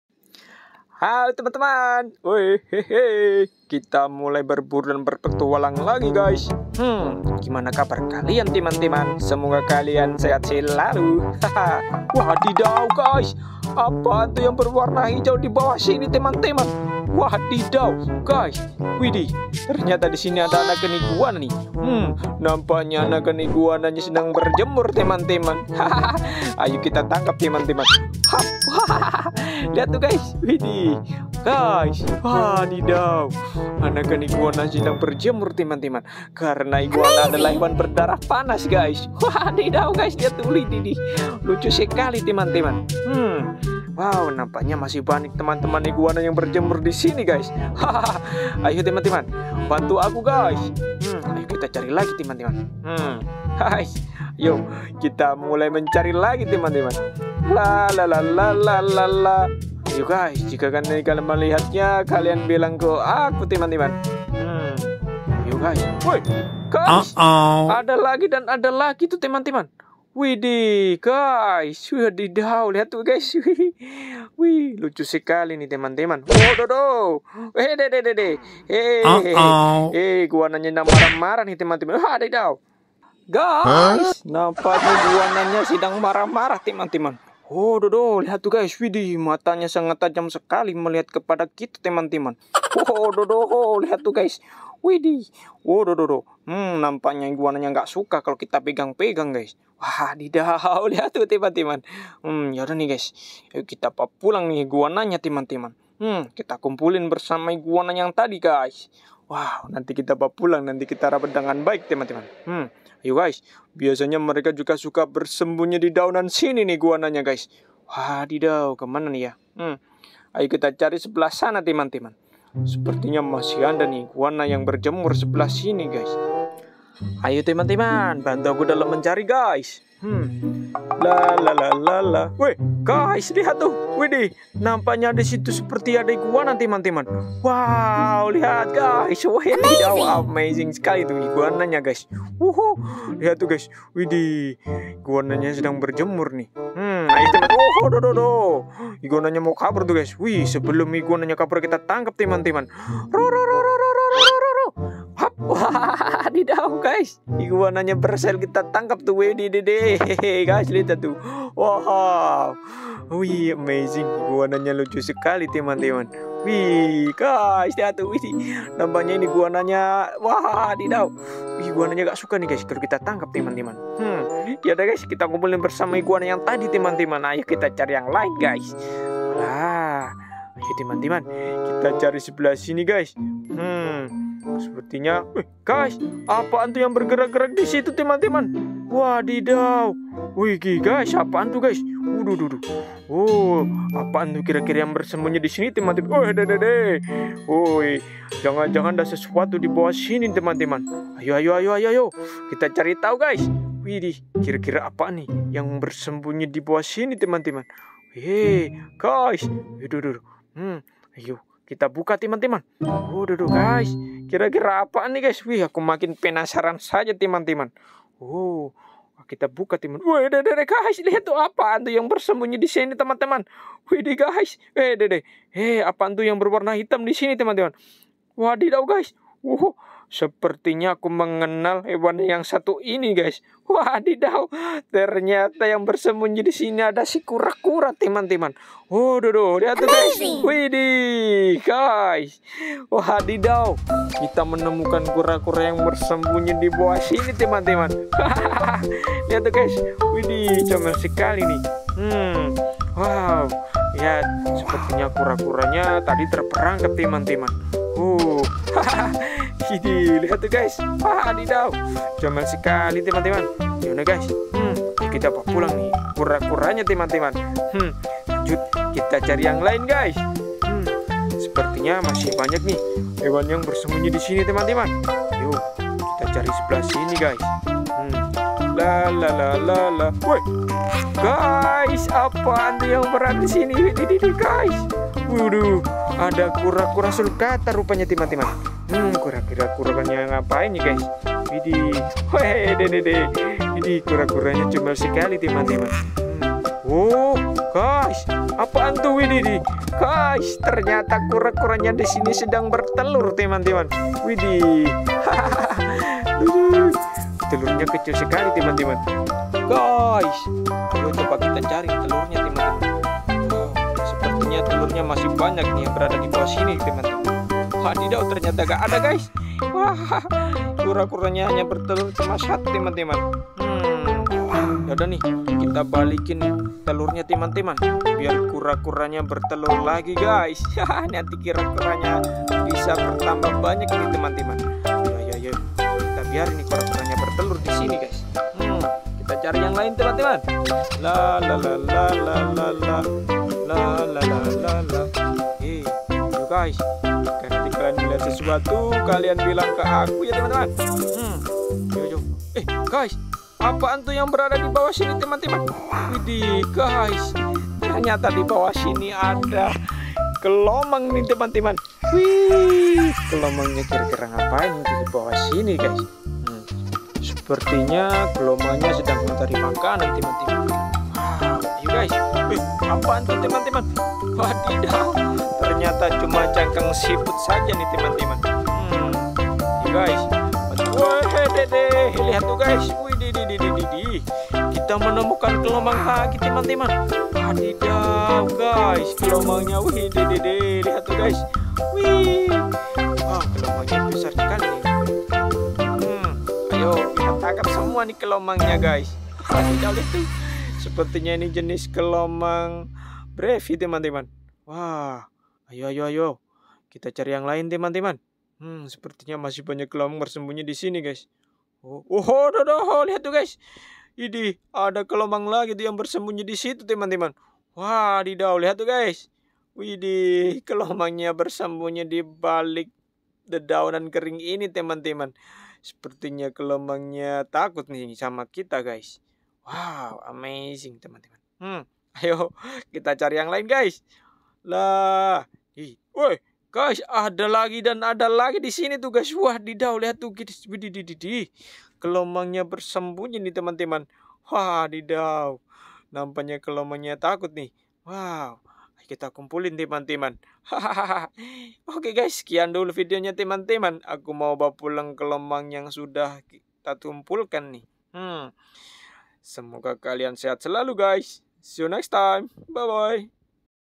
Mencari Dan Menangkap Kelomang, Kura Kura Sulcata, Iguana Hijau, Keong, Siput
ikanwarna-warni
ayamwarna-warni
kura-kurasulcata
kura-kurabrazil